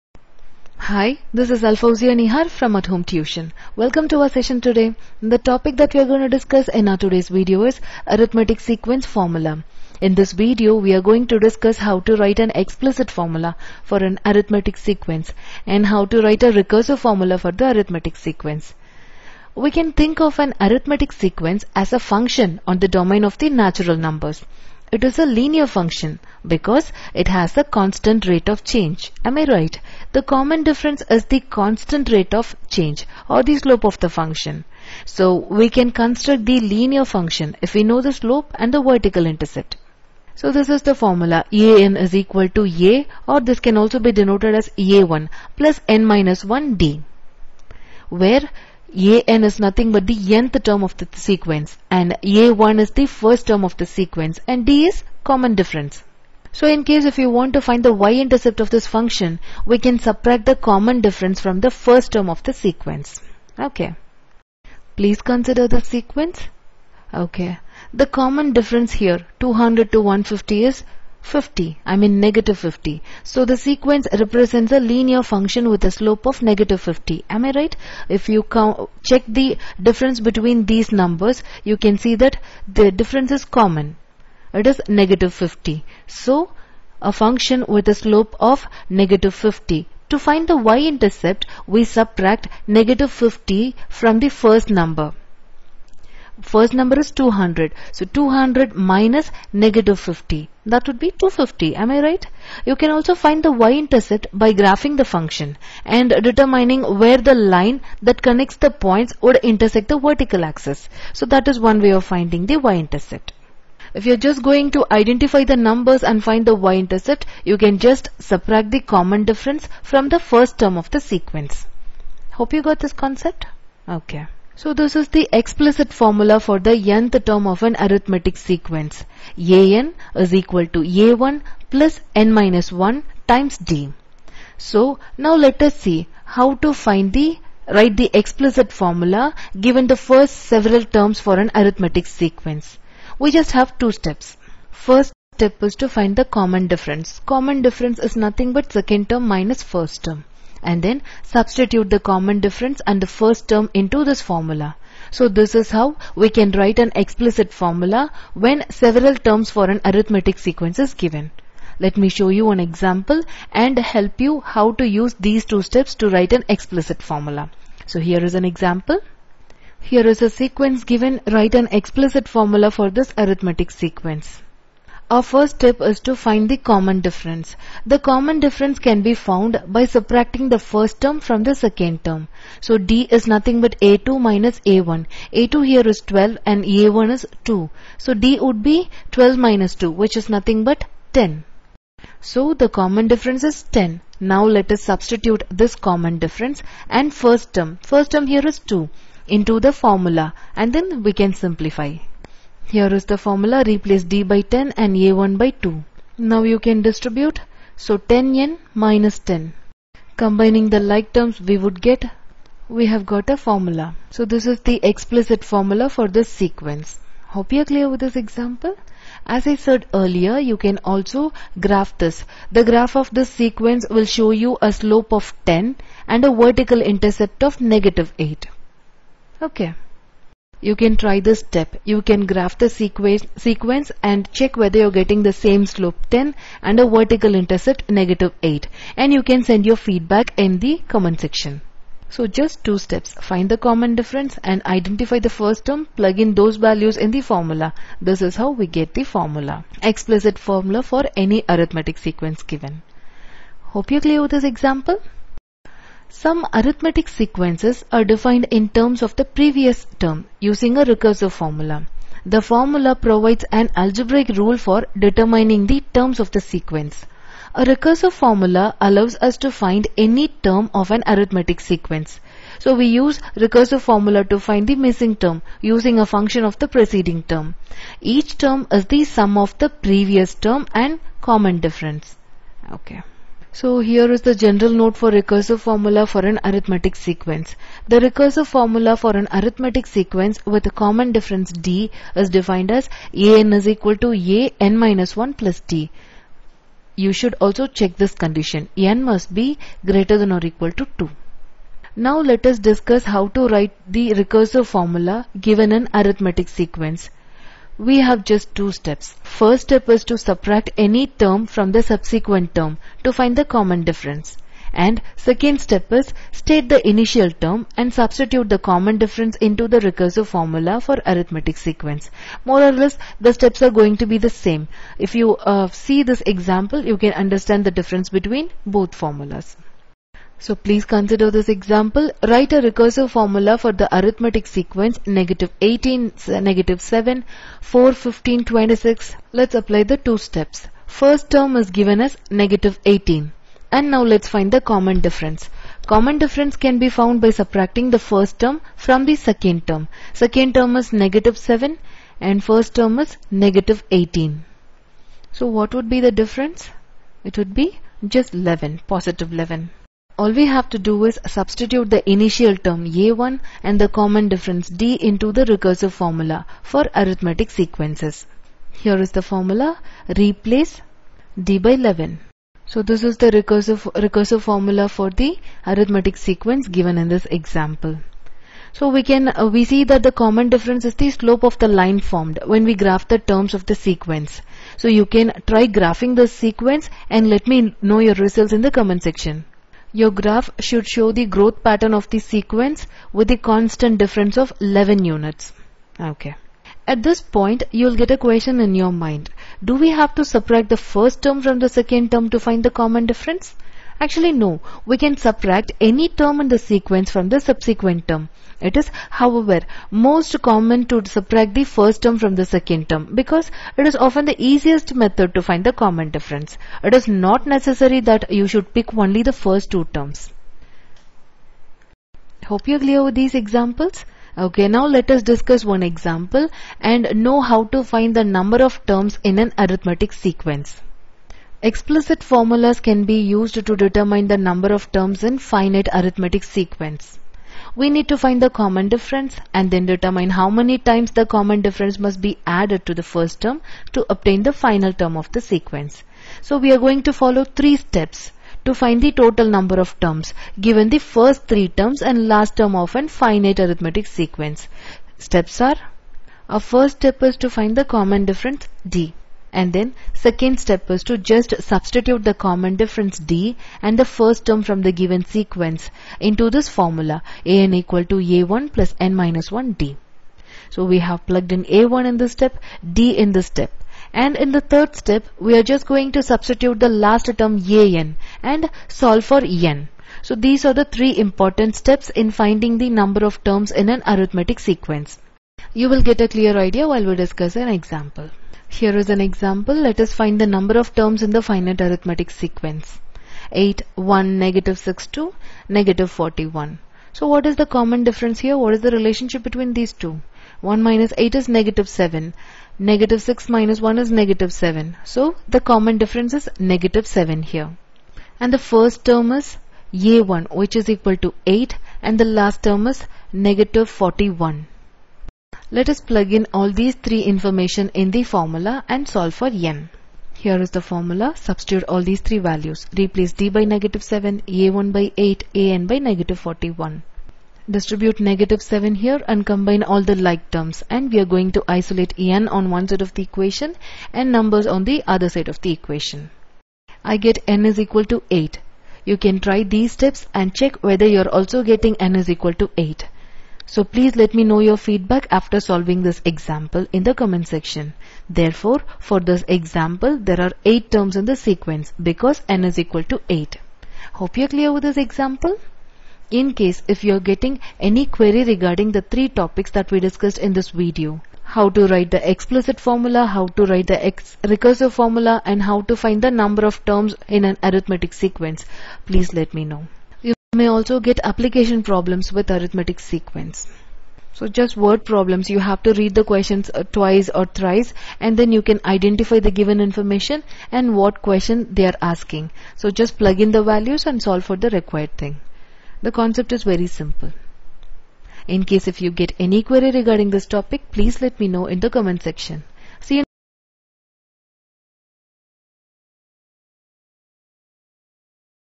Arithmetic Sequence Formula
Explicit formula
Recursive formula
How to write explicit formula for arithmetic sequence
How to write recursive formula for arithmetic sequence
Finding common difference
Finding first term
Finding last term in finite arithmetic sequence
How to find n when finite sequence is given
Number of terms in arithmetic sequence
Graph of arithmetic sequence